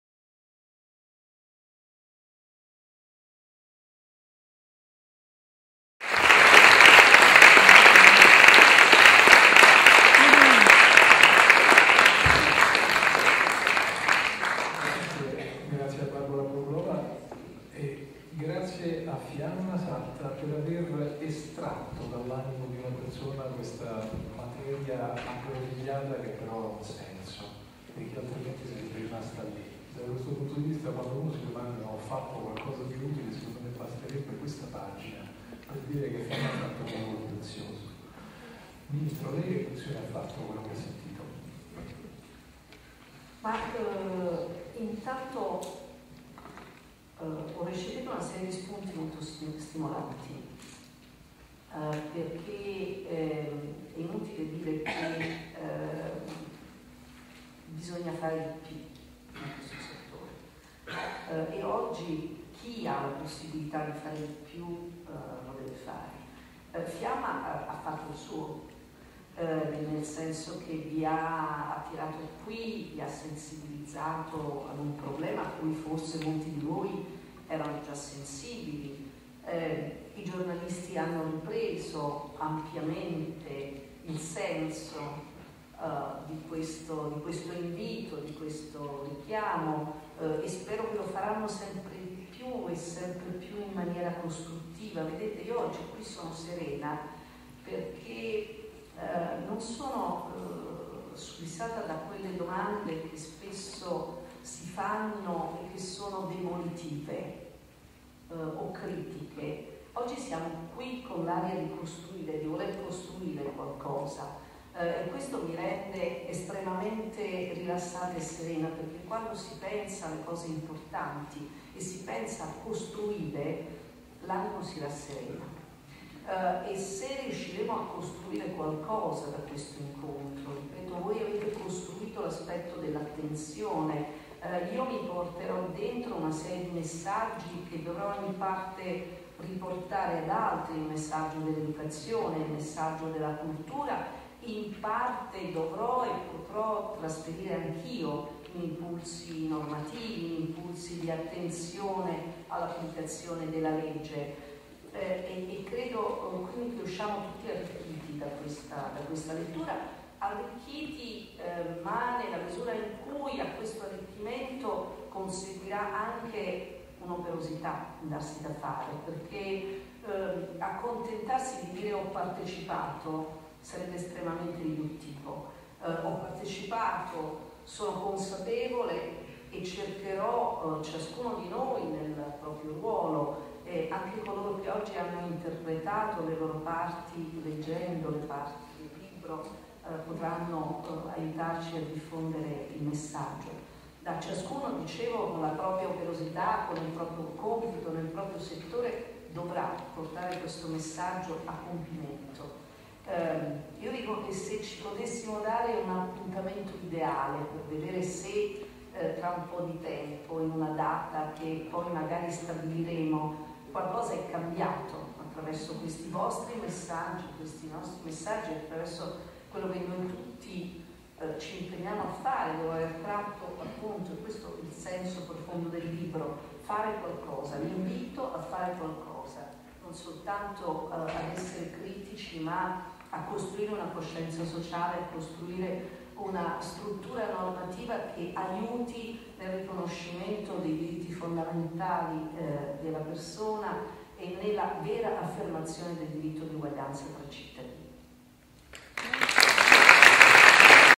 FIAMMA SATTA-MAXXI PRESENTAZIONE: Paola Severino
Presentazione del libro "Diario diversamente affabile" (Add editore) al Maxxi di Roma il 20 Giugno 2012. Con: Fiamma Satta, Maurizio Caprara, Paola Severino, Fabrizio Gifuni, Barbora Bobulova e Sergio Rubini.